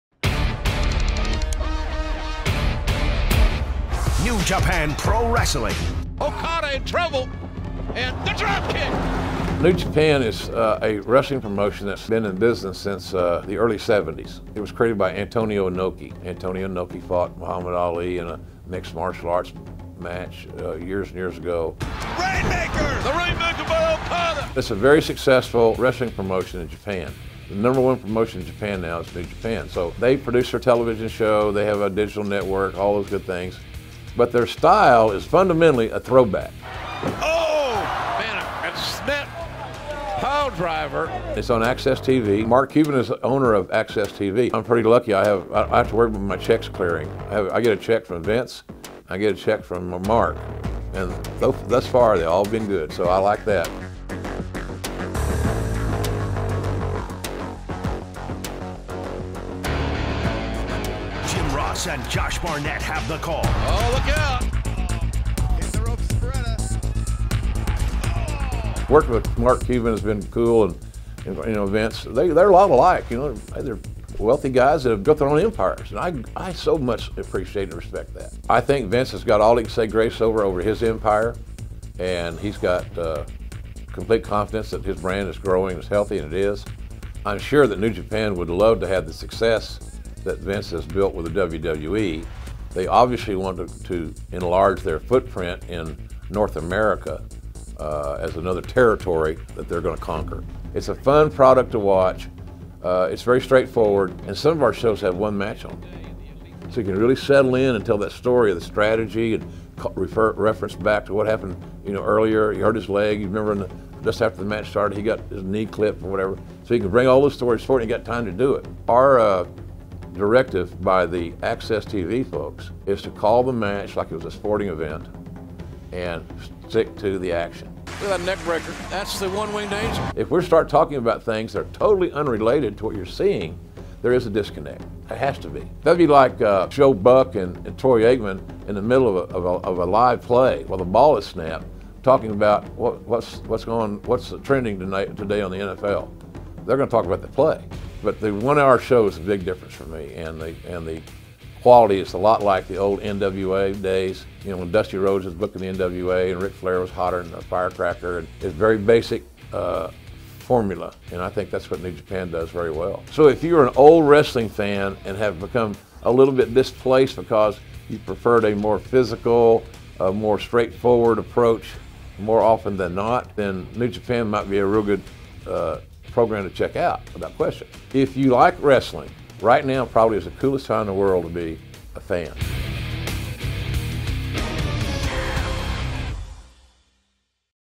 Mark Cuban está apostando fuerte a que esta compañía de lucha japonesa pueda enfrentarse a la WWE
NJPW, o "New Japan", como se la conoce comúnmente, es una promoción popular de Tokio que algunos fanáticos ven como una alternativa más intensa y físicamente más intensa a la programación de WWE.